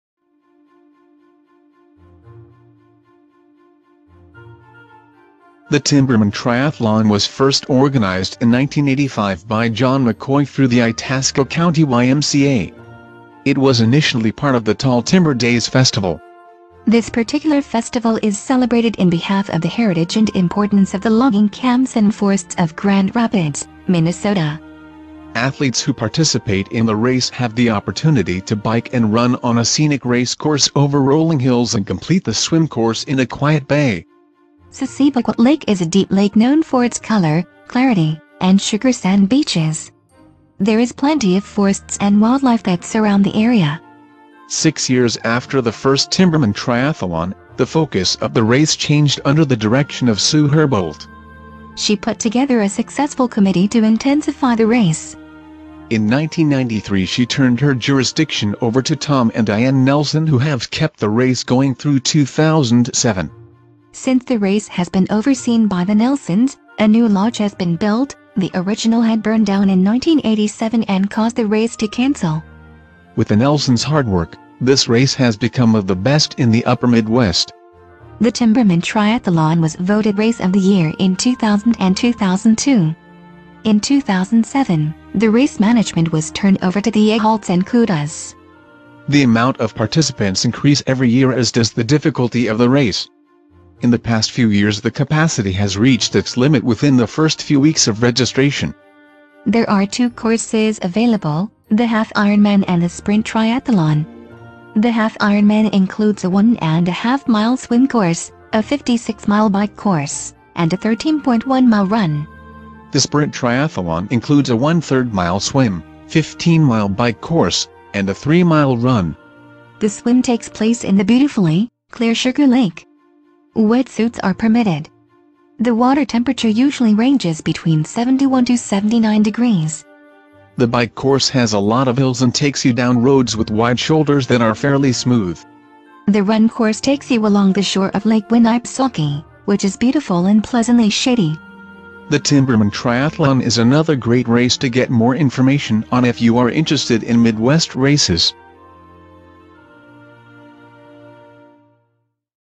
Timberman Triathlon Review
The Timberman Triathlon was first organized in 1985 by John McCoy through the Itasca County YMCA. It was initially part of the Tall Timber Days Festival.

This particular festival is celebrated in behalf of the heritage and importance of the logging camps and forests of Grand Rapids, Minnesota.

Athletes who participate in the race have the opportunity to bike and run on a scenic racecourse over rolling hills and complete the swim course in a quiet bay.

Siseebakwet Lake is a deep lake known for its color, clarity, and sugar sand beaches. There is plenty of forests and wildlife that surround the area.

Six years after the first Timberman Triathlon, the focus of the race changed under the direction of Sue Herboldt.

She put together a successful committee to intensify the race.

In 1993 she turned her jurisdiction over to Tom and Diane Nelson who have kept the race going through 2007.

Since the race has been overseen by the Nelsons, a new lodge has been built (the original had burned down in 1987 and caused the race to cancel).

With the Nelsons hard work, this race has become of the best in the upper Midwest.

The Timberman Triathlon was voted “Race of the Year” in 2000 and 2002. In 2007, the race management was turned over to the Ehalts and Koodas.

The amount of participants increase every year as does the difficulty of the race. In the past few years the capacity has reached its limit within the first few weeks of registration.

There are two courses available: the half Ironman and the sprint triathlon. The half Ironman includes a 1 ½ swim course, a 56 mile bike course, and a 13.1 mile run.

The sprint triathlon includes a 1/3 mile swim, 15 mile bike course, and a 3 mile run.

The swim takes place in the beautifully, clear Sugar Lake. Wet suits are permitted. The water temperature usually ranges between 71 to 79 degrees.

The bike course has a lot of hills and takes you down roads with wide shoulders that are fairly smooth.

The run course takes you along the shore of Lake Winnipesaukee, which is beautiful and pleasantly shady.

The Timberman Triathlon is another great race to get more information on if you are interested in Midwest races.